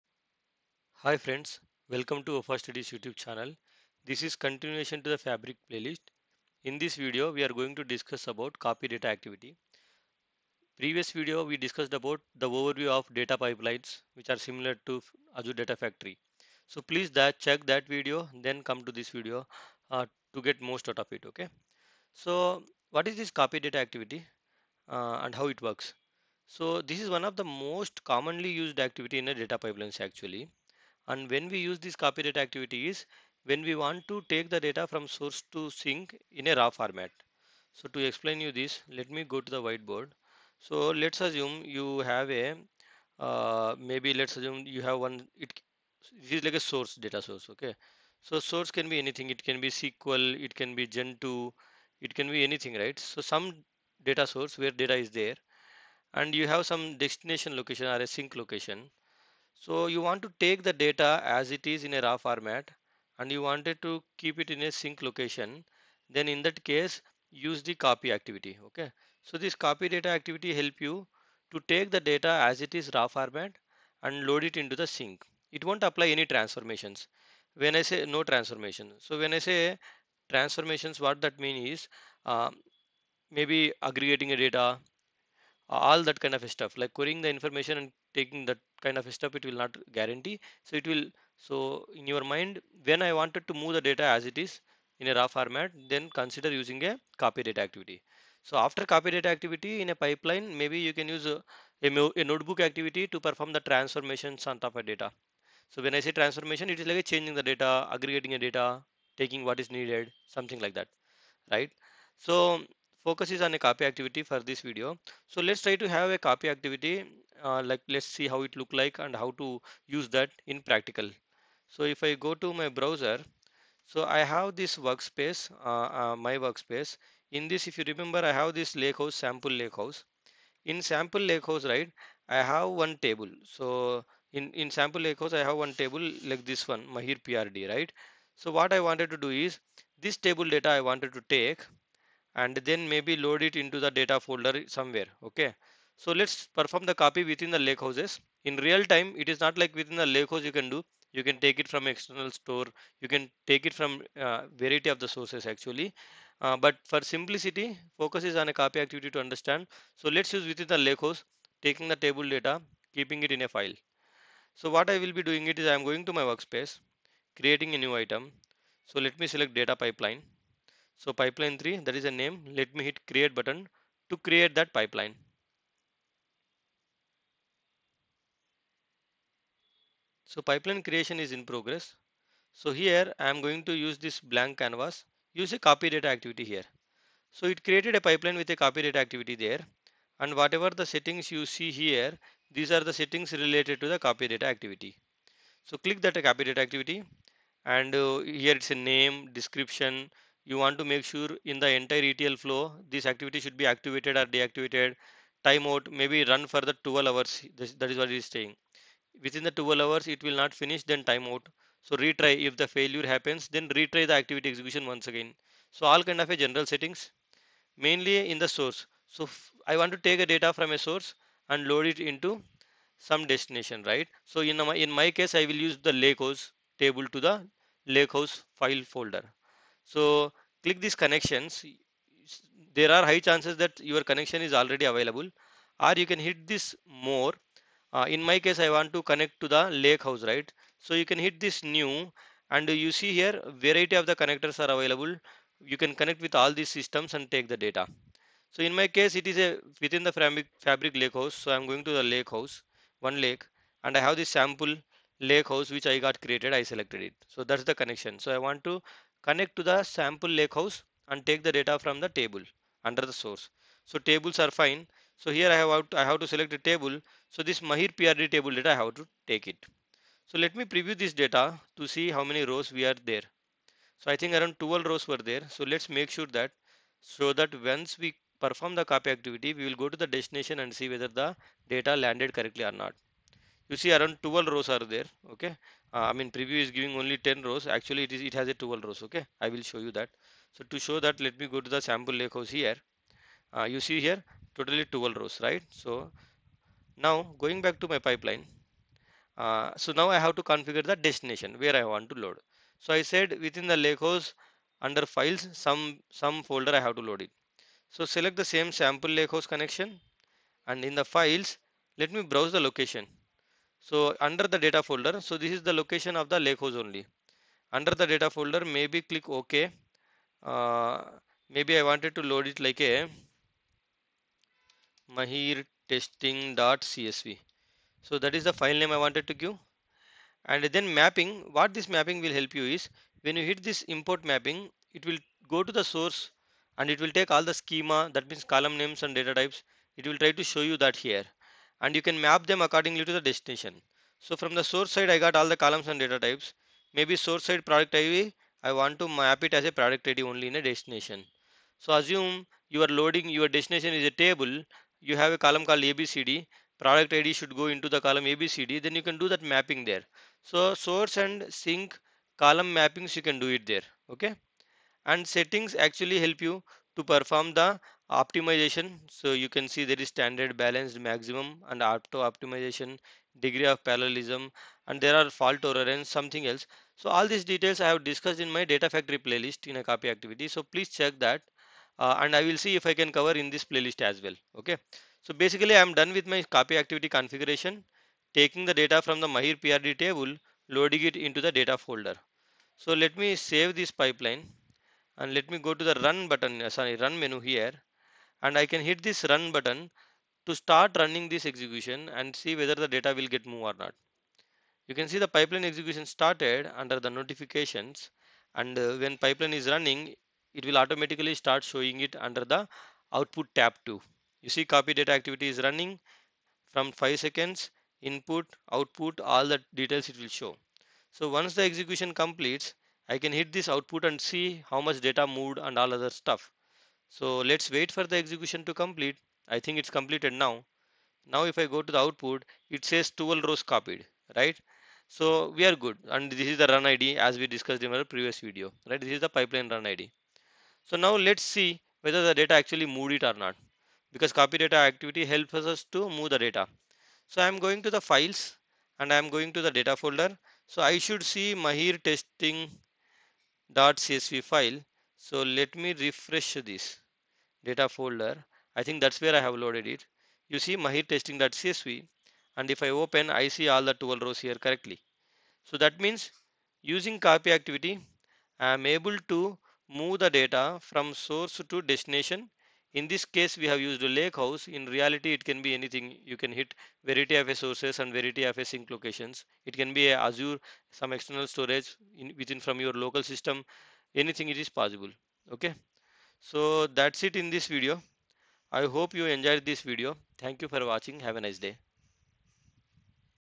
14. Copy Data Activity in Data Pipelines in Microsoft Fabric
In this video, I discussed about Copy Data Activity in Data Pipelines in Microsoft Fabric